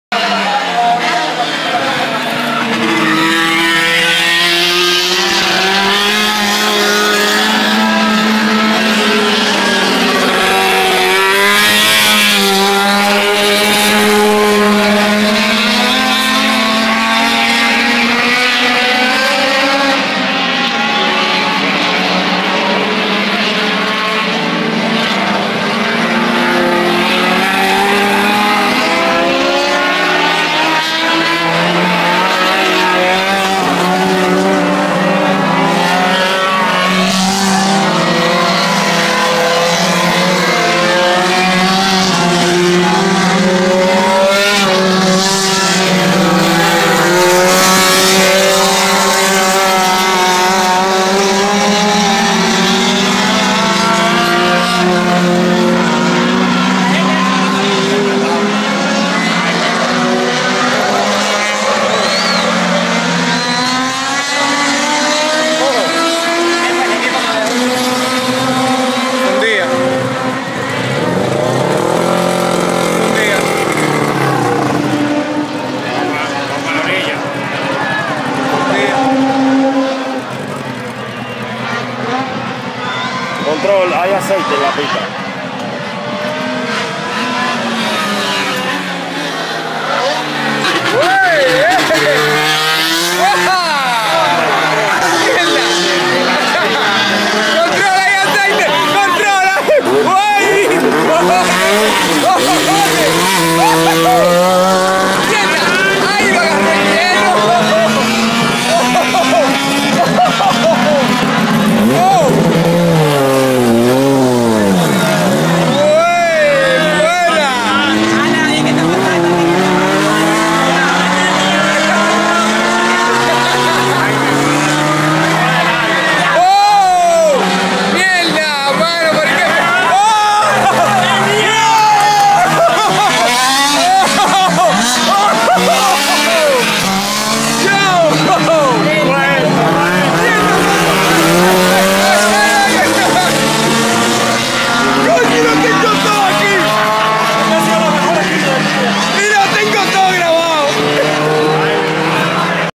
Accidente por aceite en el Autodromo movil 1 (23 de marzo 2
Accidente por aceite derramado de una Honda Civic ek a causa de fundida de motor , estos fueron los divertidos resultados